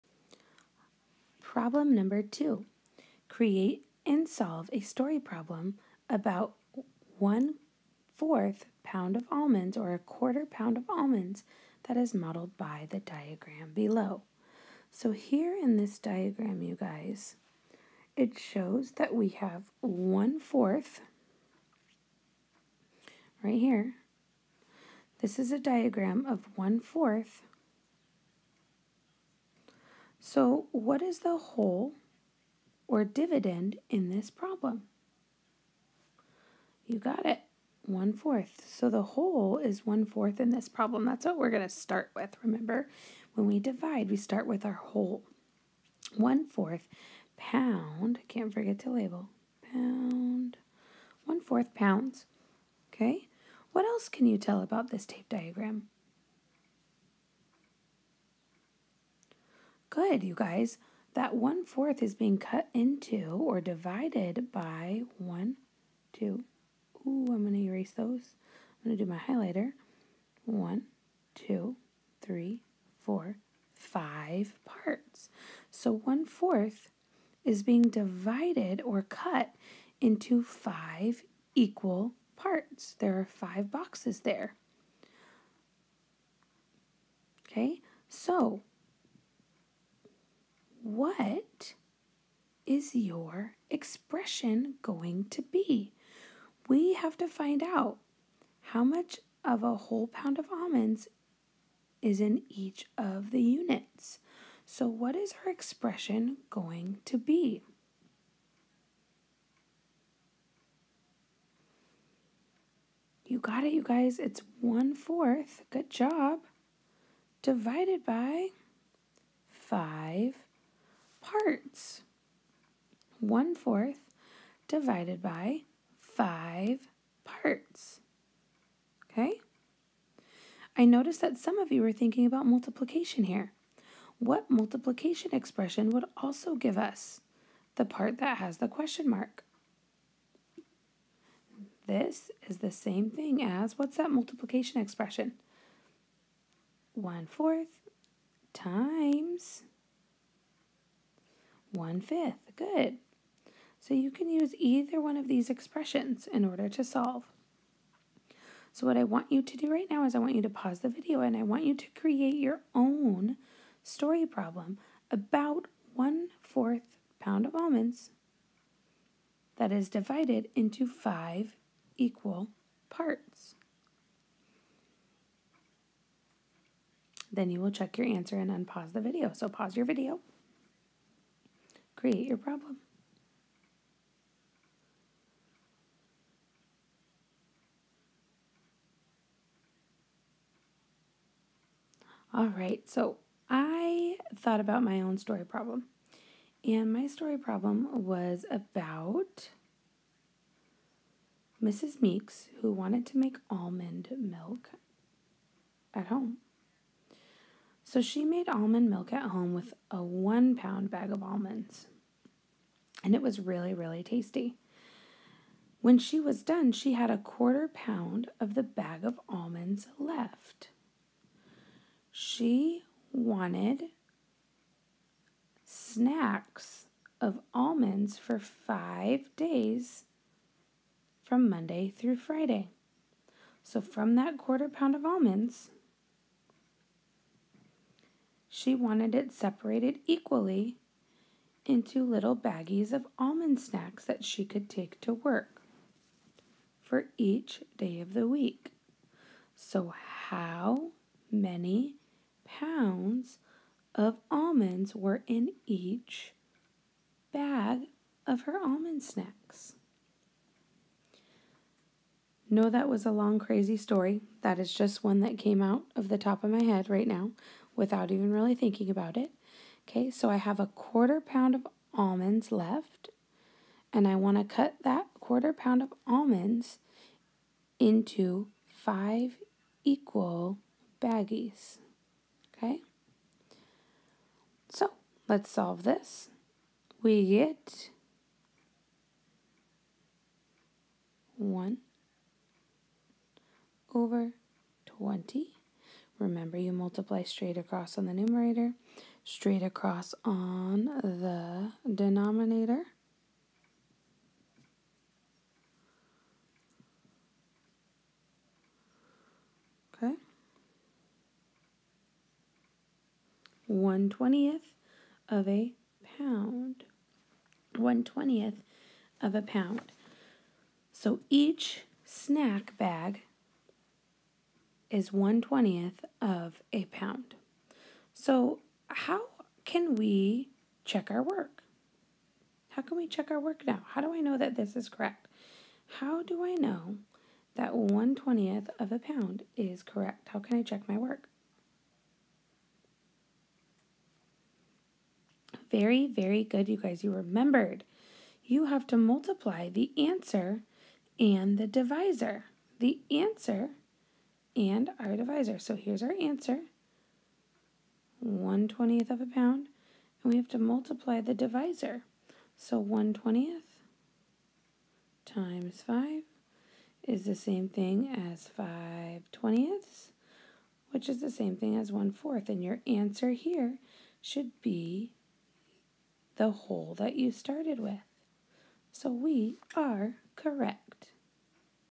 Module 4 Lesson 28 Concept Development #2
Create and solve a story problem modeled by a tape diagram.